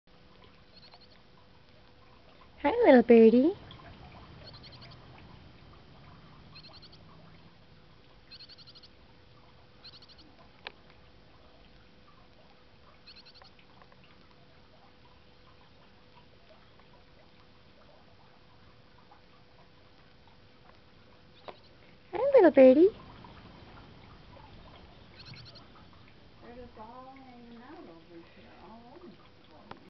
Bush Tit Bird
adorable little Bush Tit that hopped into my hand today when I found him out int he backyard. He stayed there for a good 15 minutes until I finally had to nudge him onto the hedge :)